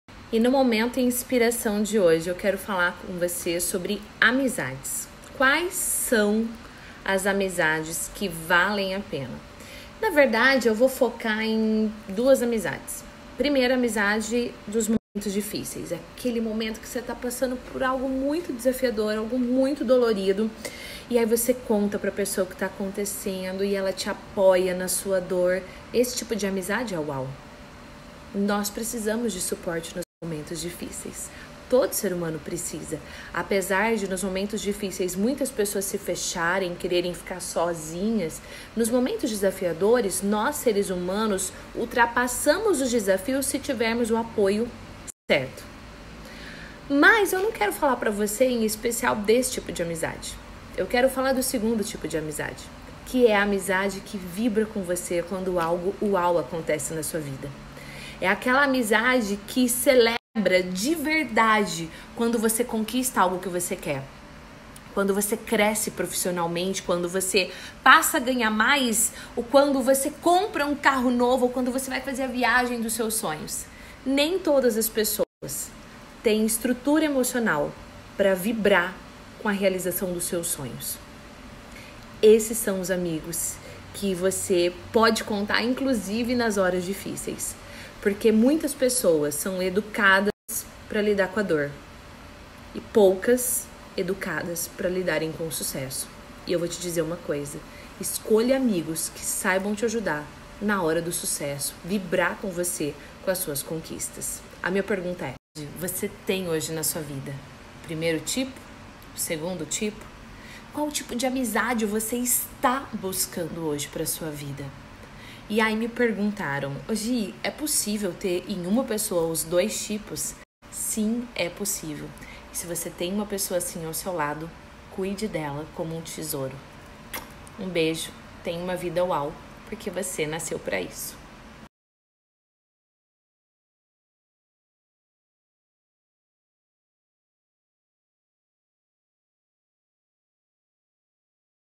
Como fazer amigos | Qual tipo de amizade você tem?
Como fazer amigos | Qual tipo de amizade você tem? Você já se perguntou qual é o tipo de amizade que você tem? Ou como fazer amigos e que tipos de amigos? No vídeo de hoje eu te convido a refletir sobre amizade e amigos, tipo de amigo e que mais importante do que saber como ter amigos, como conquistar amigos ou como conquistar amigos e influenciar pessoas é saber qual tipo de amigo você tem por perto.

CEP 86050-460